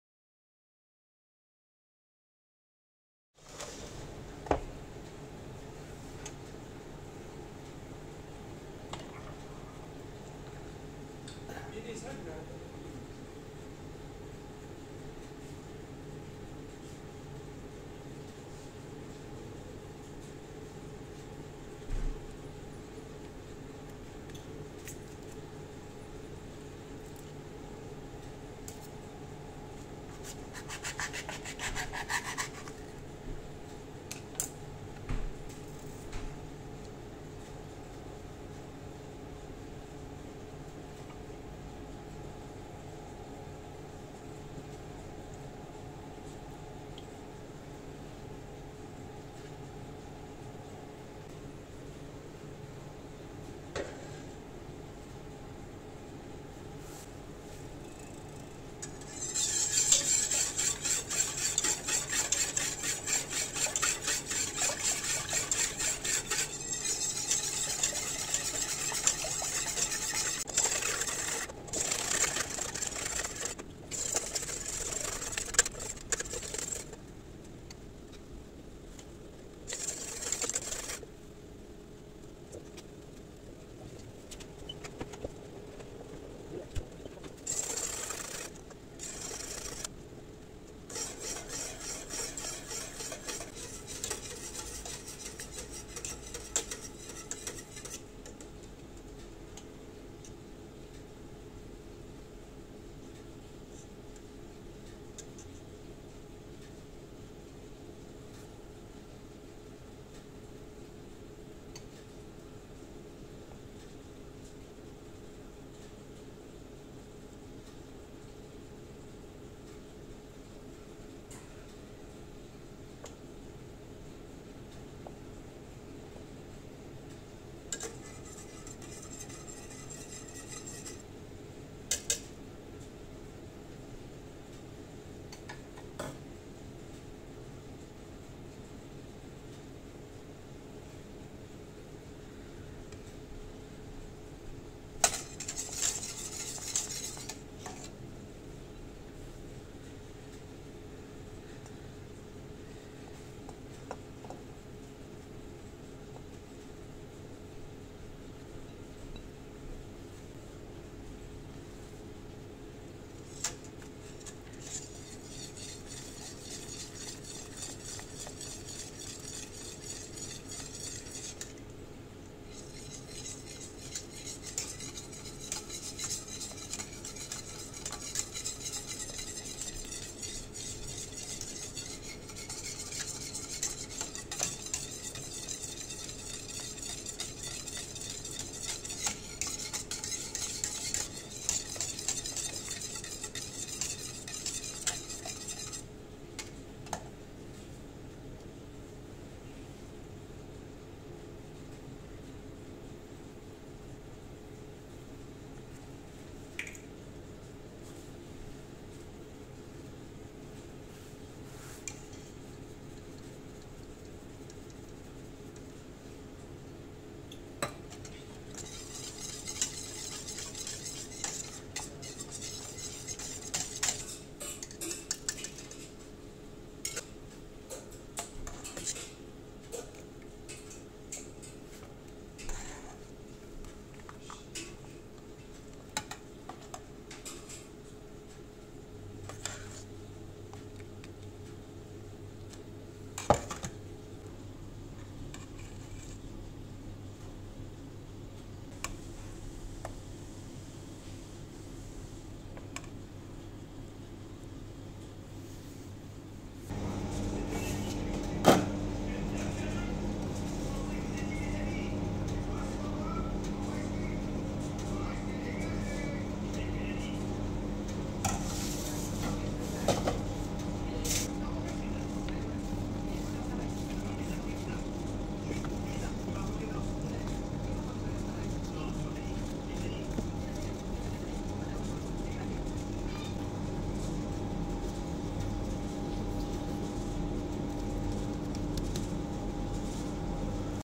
How to make cheese sauce for fast food
Hello!

friends, in this video I will prepare the cheese sauce that we put in tacos, do not forget to subscribe to the channel, and to like the video if you like.

As you know the cheese sauce is fundamental for the taco, it also goes well with many other dishes both in the restaurant and at home.

ingredients
_ 1L of Liquid cream 18% or 30%Fat
_ 100g Emmental cheese
_ 1 or 2 slices of cheddar cheese

For the procedure you can watch the video, I hope you like it 🥰🥰 I'm not very experienced in editing 😊😊☺️.